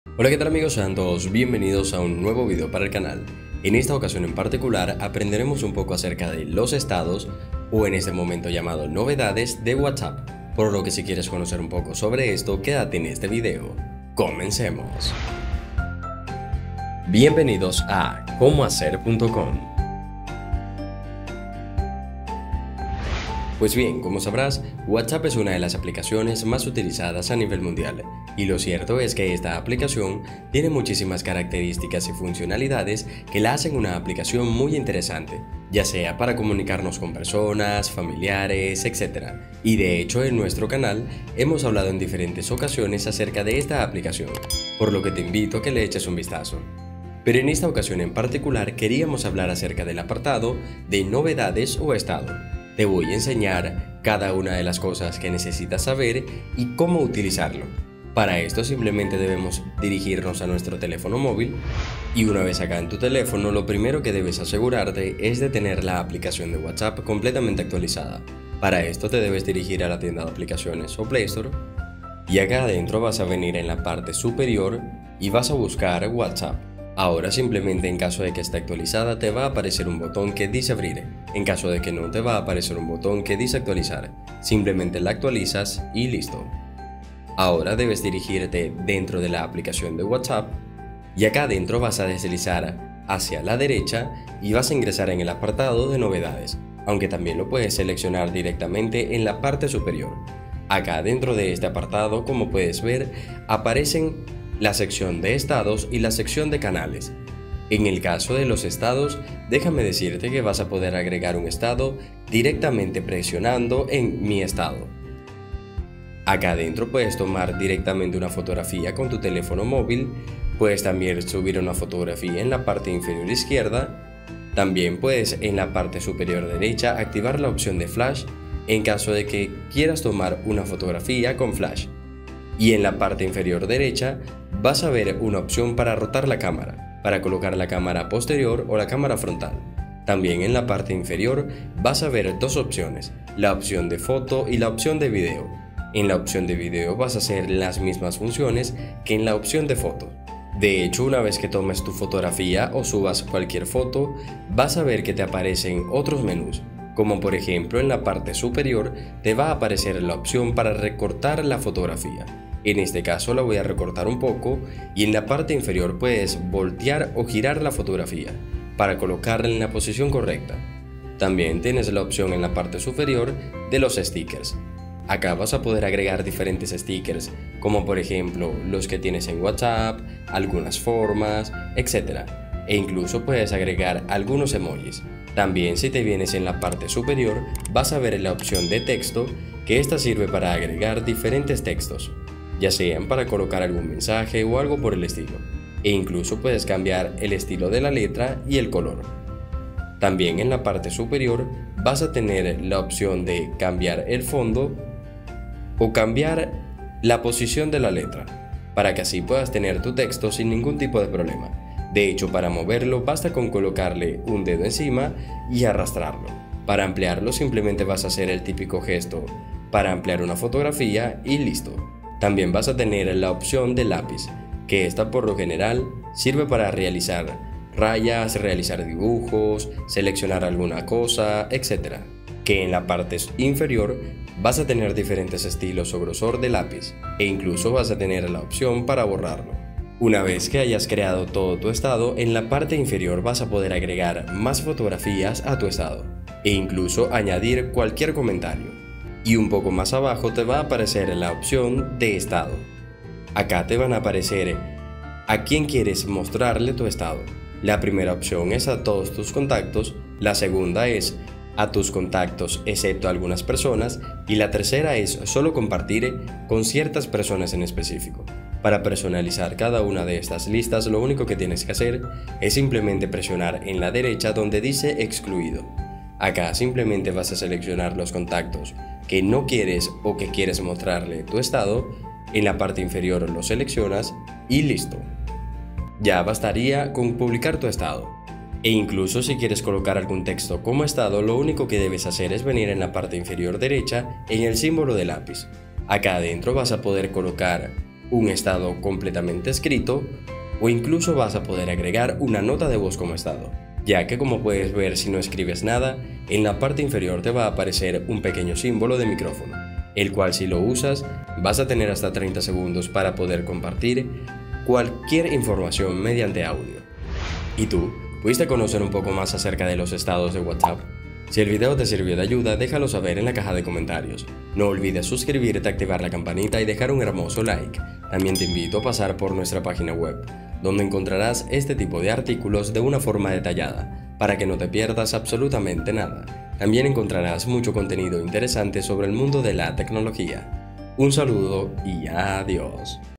Cómo Sacar el Máximo Provecho a tus ESTADOS DE WHATSAPP | 🌟 Guía y Trucos WhatsApp 🌟
En este video, te llevaré a un viaje exploratorio por el fascinante mundo de los Estados de WhatsApp. Descubre cómo elevar tu juego de actualizaciones de estado a un nuevo nivel y sacar el máximo provecho de esta función para expresarte, conectarte con amigos y añadir un toque único a tu presencia en línea.